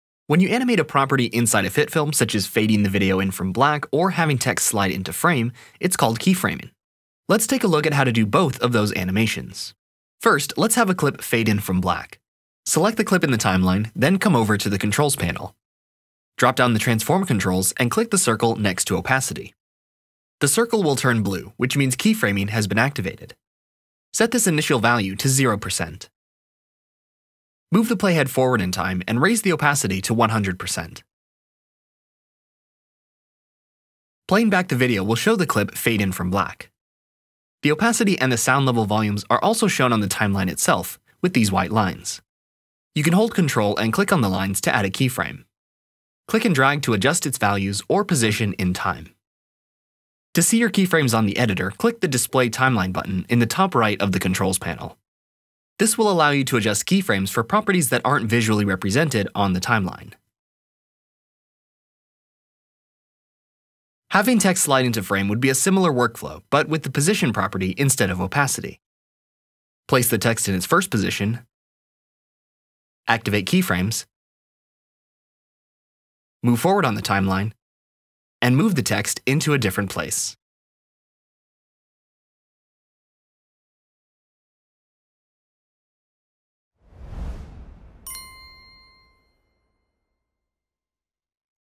How to add animation in HitFilm | Enhance Your Videos
Animating parts of your footage can help turn a boring cut into something a lot more engaging for your viewers.